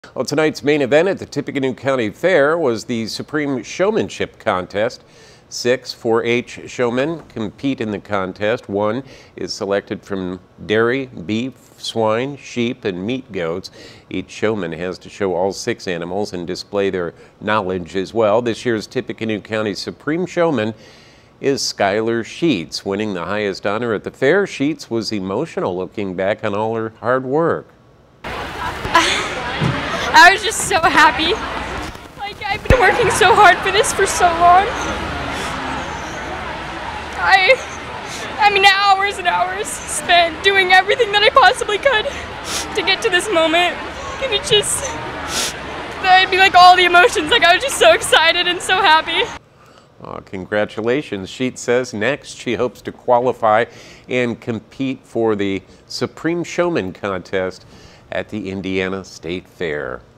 Skylar Sheets Wins Supreme Showmanship
4-H member Skylar Sheets is your 2023 Supreme Showmanship winner at the Tippecanoe County 4-H Fair